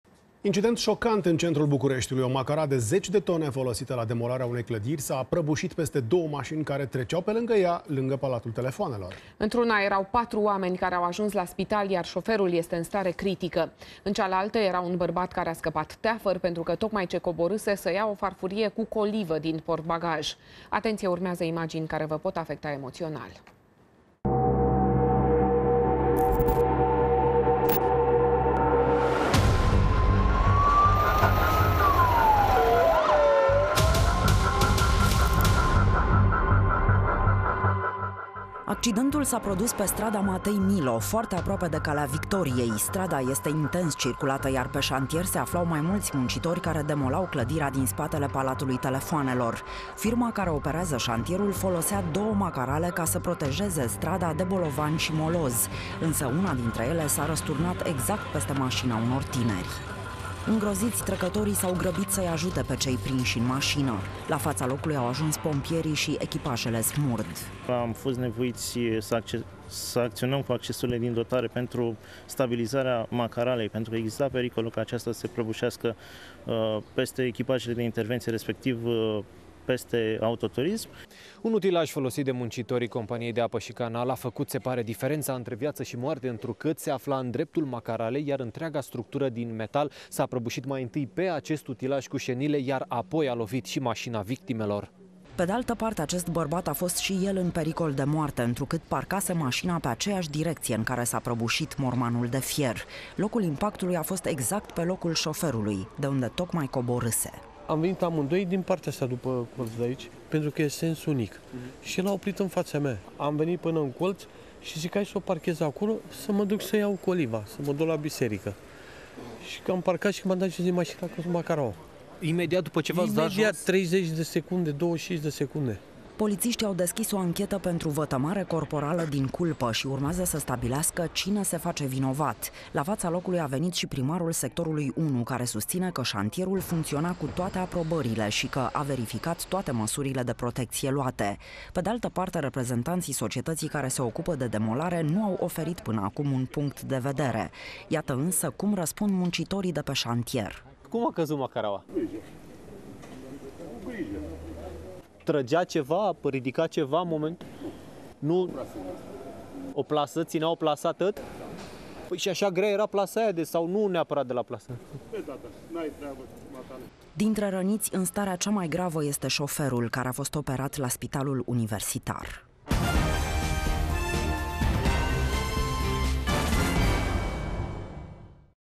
Șoferul mașinii zdrobite de macara este în stare critică. Cum a scăpat teafăr al doilea șofer
Incident șocant în centrul Bucureștiului: o macara de zeci de tone, folosită la demolarea unei clădiri, s-a prăbușit peste două mașini aflate în trecere, lângă Palatul Telefoanelor.